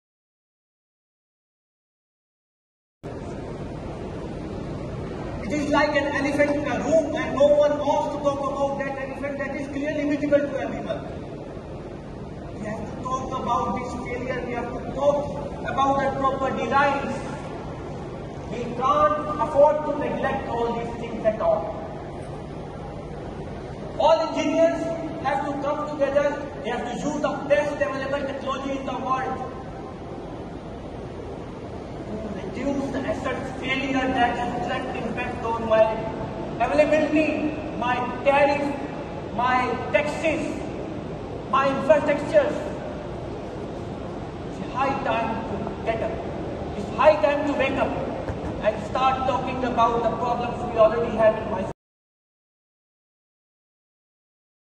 Don't Ignore Elephant in the Room - Let's Make India Safe
We should not ignore the elephant in the room... let's work together to make India free of Electrical and Fire 🔥🔥 Accidents